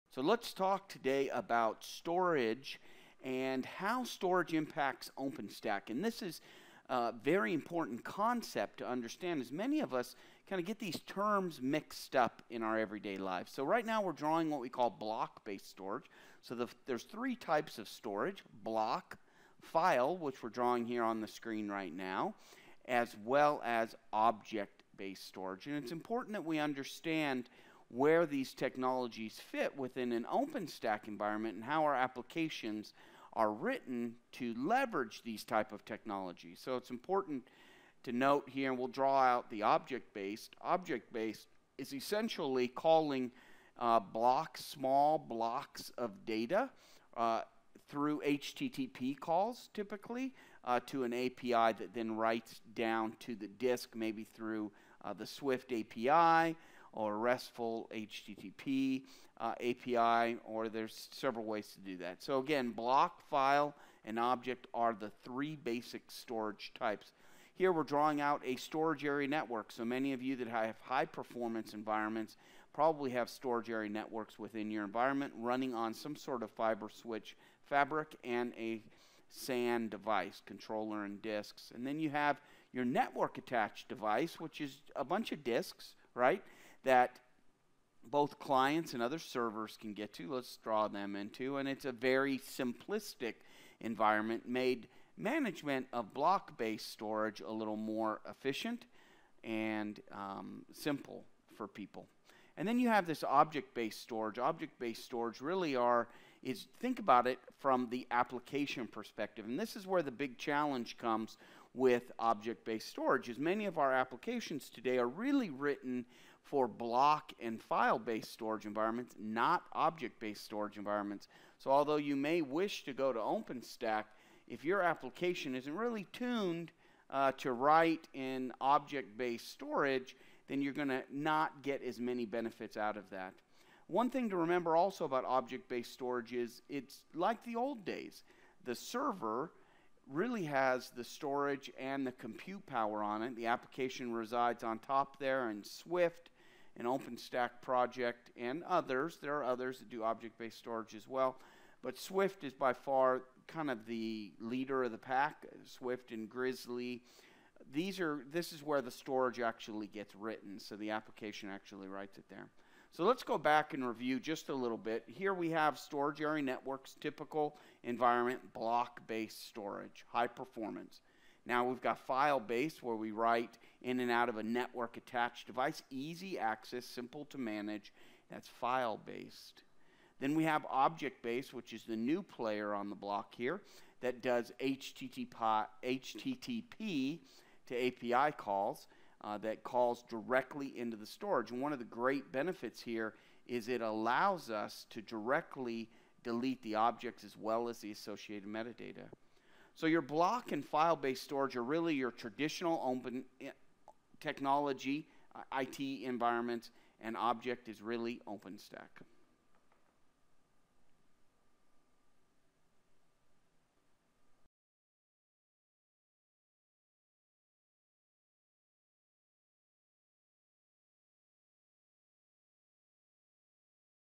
Openstack and Storage simplified
Explore the differences between Block, File and Object storage and how it relates to OpenStack